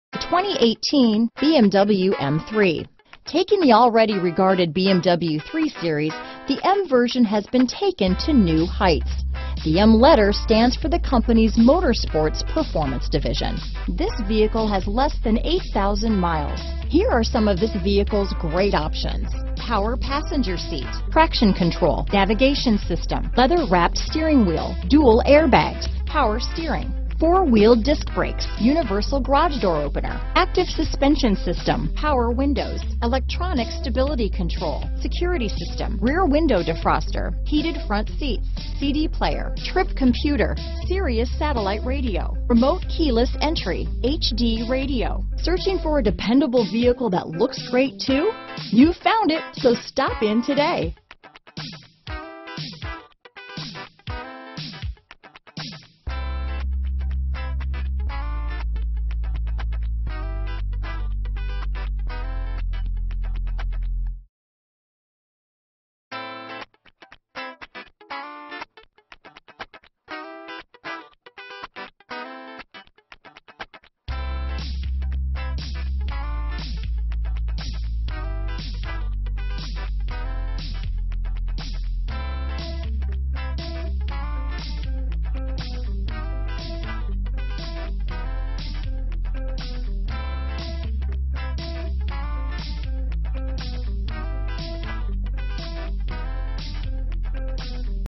2018 BMW M3 at Oxmoor Toyota | Louisville & Lexington, KY T46140A
Austin Yellow Metallic U 2018 BMW M3 available at Oxmoor Toyota in Louisville, Kentucky. Servicing the Lexington, Elizabethtown, KY New Albany, IN Jeffersonville, IN area.

Oxmoor Toyota
8003 Shelbyville Rd

** RARE and HARD TO FIND **, ** LOCALLY OWNED **, ** BLUETOOTH **, ** REAR BACKUP CAMERA **, ** SUPER CLEAN INSIDE AND OUT **, ** ONLY ONE OWNER **, ** CLEAN CARFAX HISTORY REPORT **. CARFAX One-Owner.brPriced below KBB Fair Purchase Price!